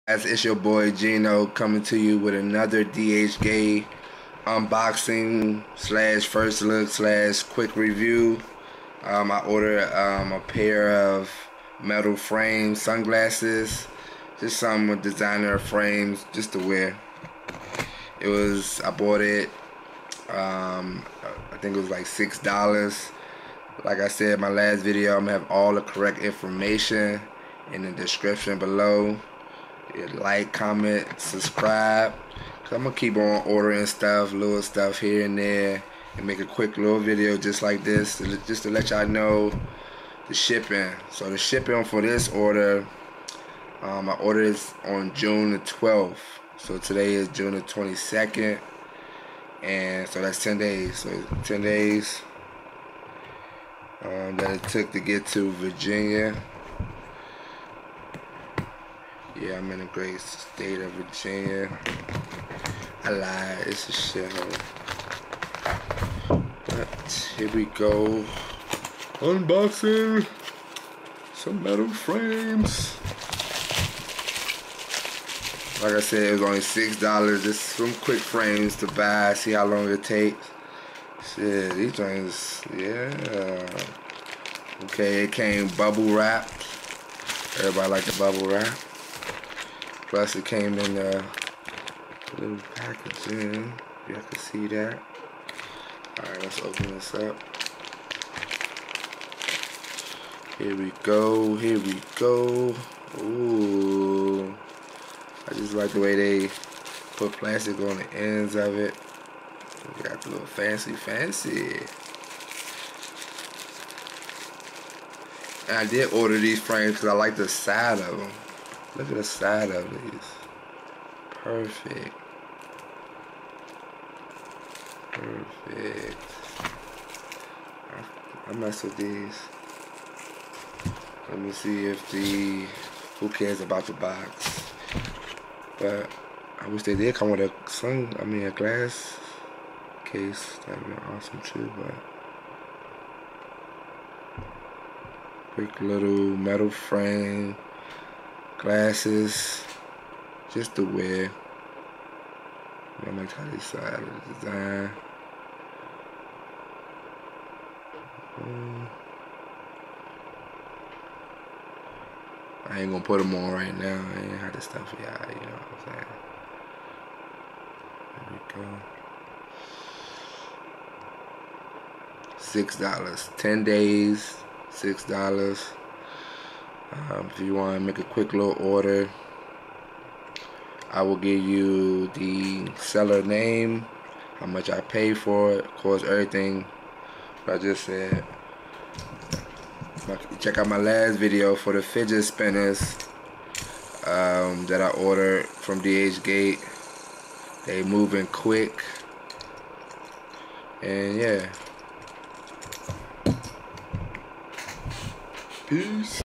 DHGATE SUNGLASSES UNDER $7!!
Optical Round Metal Frame Sunglasses

Seller: Minchien
Price: $6.15each
Shipped to Virginia in 10 days